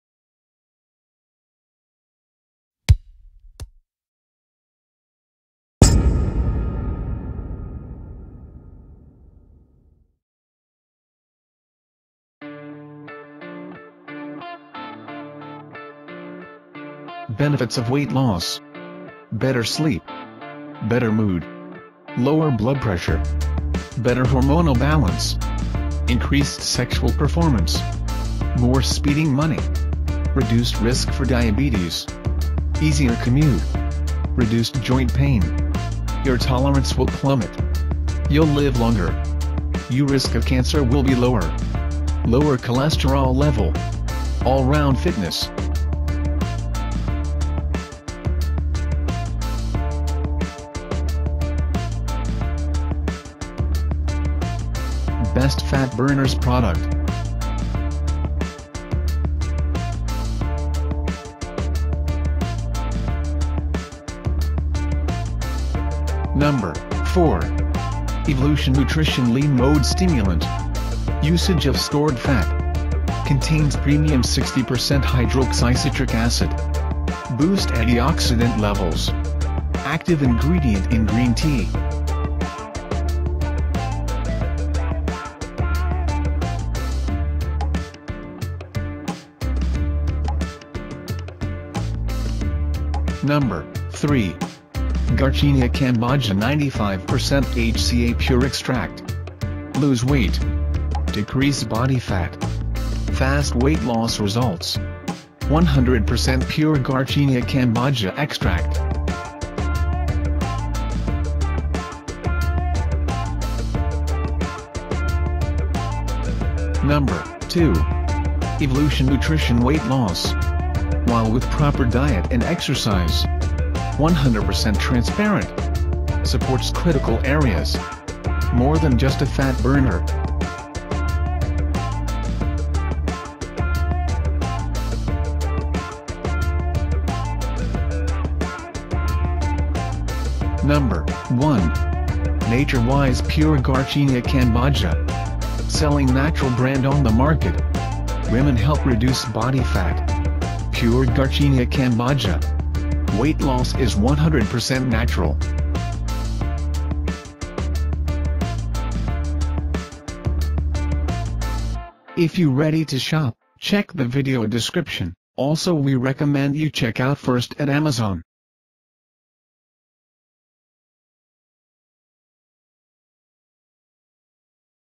Top 4 Best Fat Burners Product Reviews 2018 | Benefits of losing weight
Do you know about Benefit of Weight loss. If you want to reduce your weight with any exercise you can used this product.

Evlution Nutrition Lean Mode Stimulant

Garcinia Cambogia 95% HCA Pure Extract with Chromium.

Evlution Nutrition Weight Loss Trans4orm

NatureWise Pure Garcinia Cambogia

Another Product Reviews Below :

😍Eye bags Treatment Baebody Eye Gel for Dark Circles😀

😇Nizoral Anti Dandruff Shampoo Reviews😇